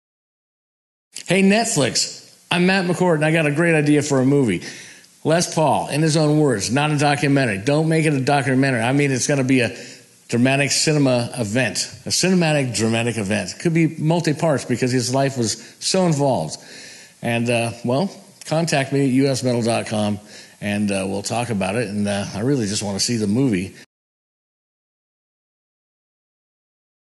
:es Paul the most important man in music technology r=recording and guitar
ater a few years of sending emails to many people and getting nowhere i dont know how to pitch a movie so i am doin it this way- my "iIn his own words" a book a bio on les by les ,--- the script is all there! it simply needs to be adapted for a screenplay so many dramatic events many historical milestones and he lived a long life! it could be a multi part series on netflix like the "Hollywood" mini series everybody needs to know about the this man and his contributions not a documentary it has so much story it needs to be a dramatic movie like ray or that thing y9ou do or documentaries speak to the audience who already knows drama captures everyone
come on big tomers  make tis movie for all of uus